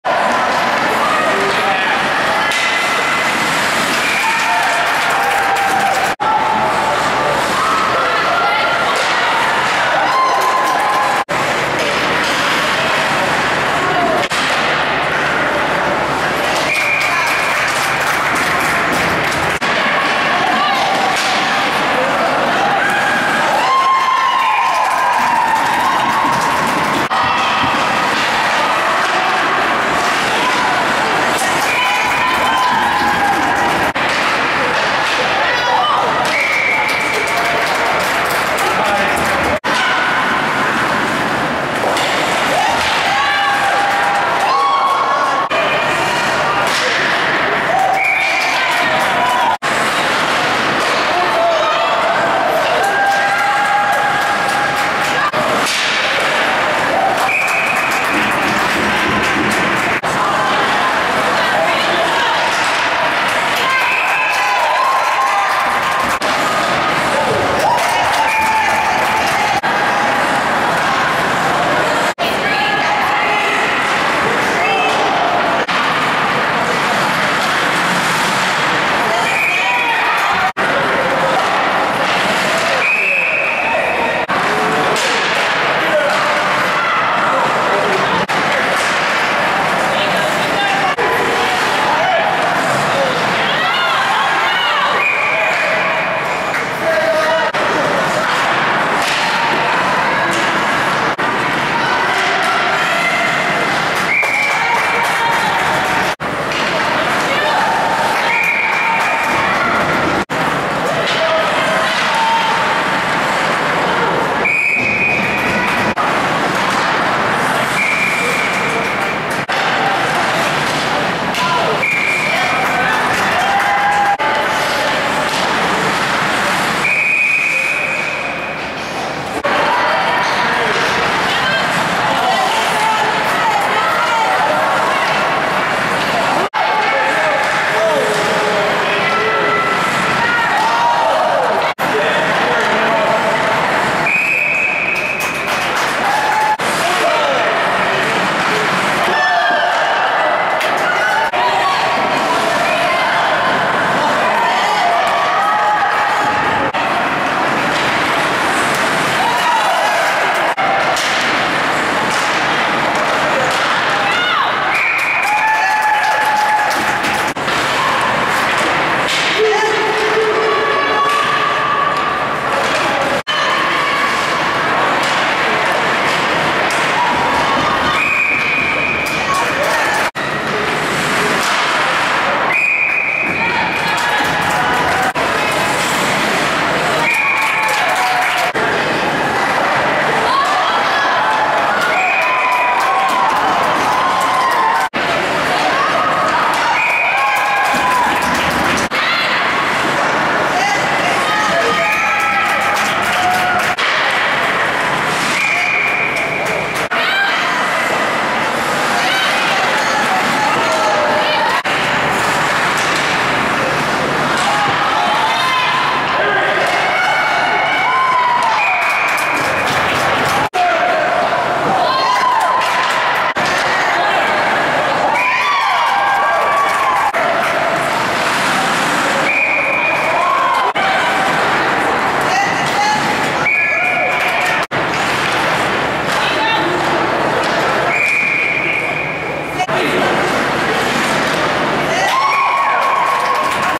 Mckenna Manalli 50 saves vs. Loyola 10-27-13
Warren girls varsity goalie Mckenna Manalli's 50 save game! Great game Mckenna!!!